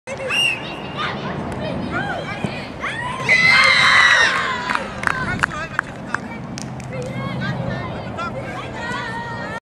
Leonidas F3 - Hermes DVS F2 (25-05-2013)
De vierde tegen Leonidas was de laatste treffer van dit seizoen, dat mooi wordt afgesloten met een verdiende 1-4 overwinning.
Een fragment uit de wedstrijd Leonidas F3 tegen Hermes DVS F2 op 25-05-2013.
De wedstrijd werd gespeeld op Leonidas te ROTTERDAM en eindigde in een 1 - 4 winst voor Hermes DVS F2.